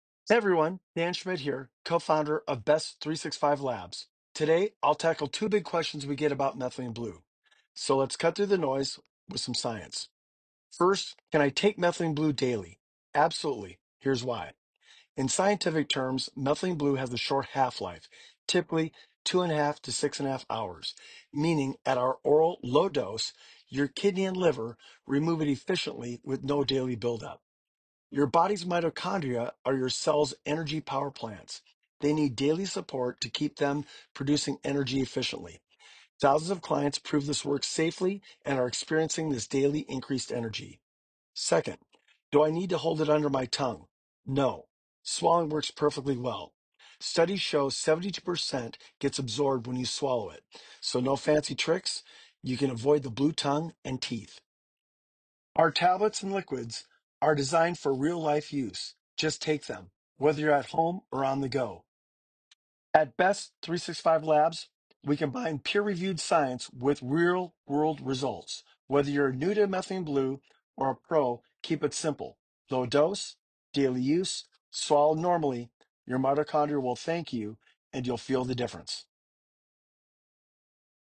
Two big questions answered about how often and when you should use methylene blue
Confused about methylene blue? Let’s clear things up:
👉 Daily use? Yes, it’s safe at low doses and helps power your cells.
👉 Swallow or hold under your tongue? Swallowing works perfectly—no blue teeth needed!
Keep it simple. Keep it effective. Your mitochondria will thank you. 🧬✨